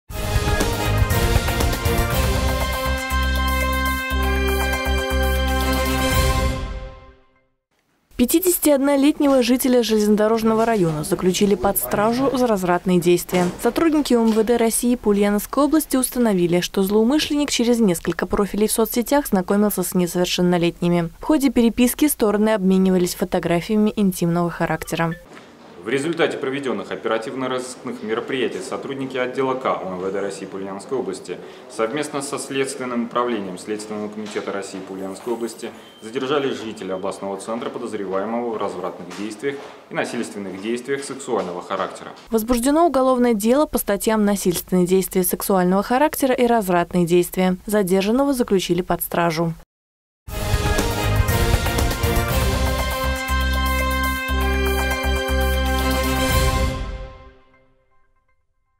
Ульяновец подозревается в развратных действиях
51-летнего жителя Железнодорожного района заключили под стражу за развратные действия.